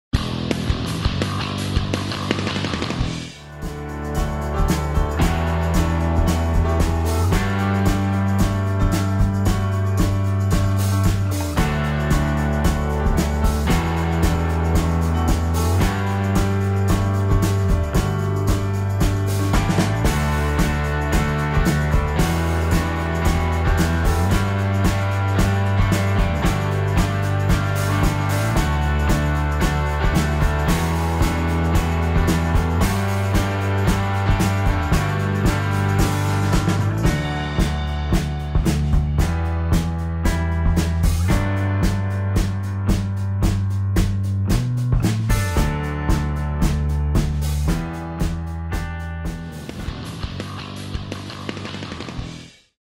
[종로이슈] 제74회 식목일 나무심기 행사
제 74회 식목일 나무심기 행사 인왕산 일대 주민 300여 명 5000그루 나무 심기